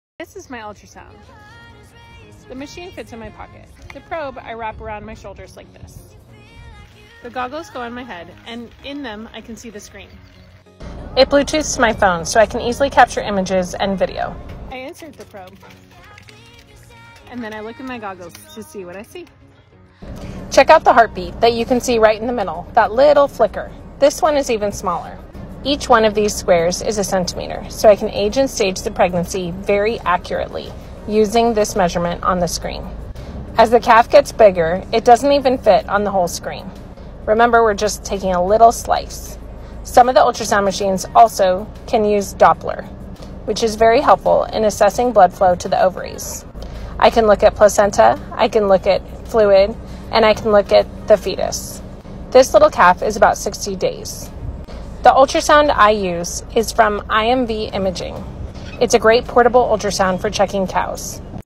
How I Use a Portable Ultrasound on Dairy Cows
Ever wondered how a portable ultrasound works on a farm? In this video, I show how I use mine to check on cows for pregnancy and overall health. Portable tools like this help me provide the best care for the herd.